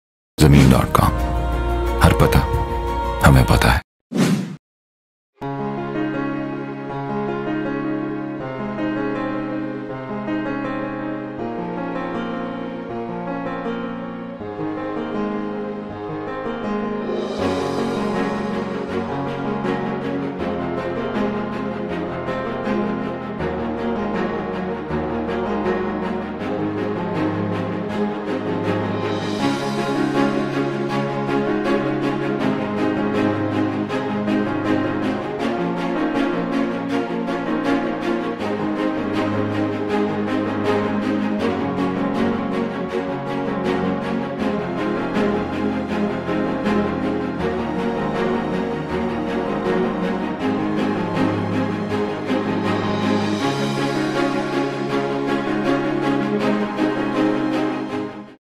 1.5 KANAL HOUSE FOR SALE IN CANAL GARDEN FAISALABAD
A nicely build 29.2 Marla House with Basement is available for sale in Canal Garden, Faisalabad.

Features:
8 Bedrooms with Attach bath
2 TV Lounge
2 Kitchen
2 Lawn
2 Gates
3 Stores
Wide Garage.